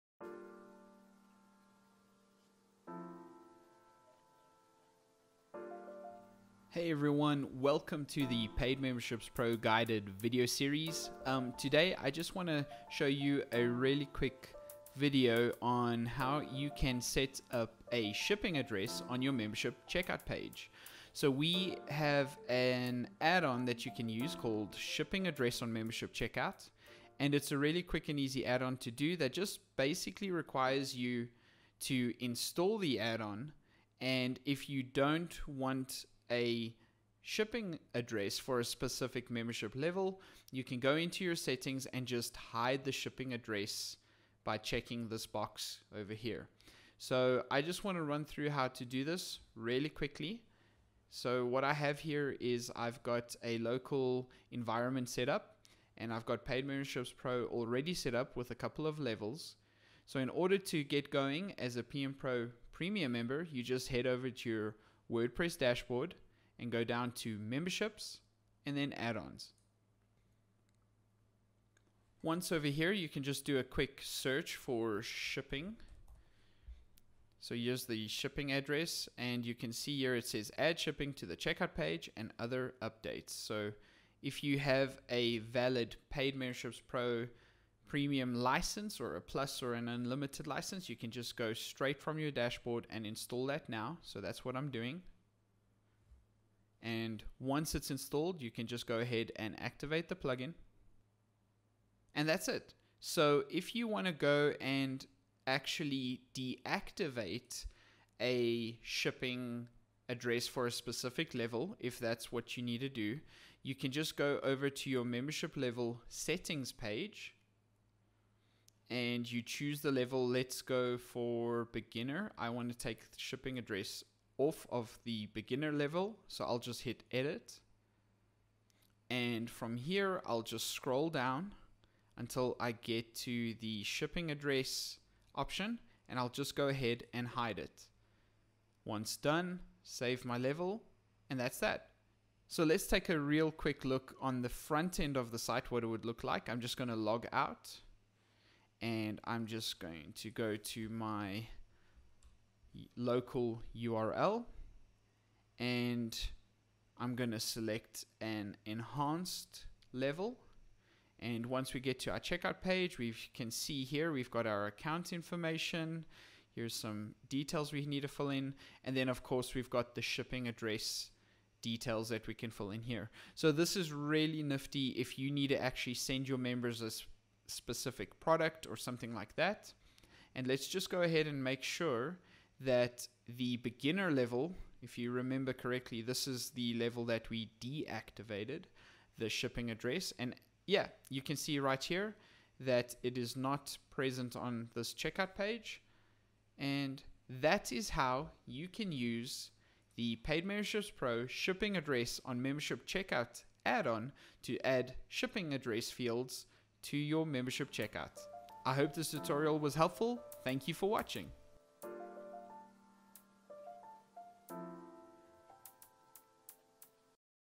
Mailing Address (formerly Shipping Address) Add On | Paid Memberships Pro
Adds Mailing Address fields to the membership checkout page, confirmation page, confirmation emails, member’s list, and edit user profile pages.

By default, every level will now have a “Mailing Address” section on the Membership Checkout page. If you would like to display this section only for certain levels:

1) Navigate to Memberships - Membership Levels - Edit Level.
2) Under the heading “Mailing Address” check the box to hide the fields for this level.
3) Save Settings

(Repeat for each level that should not display the “Mailing Address” section.)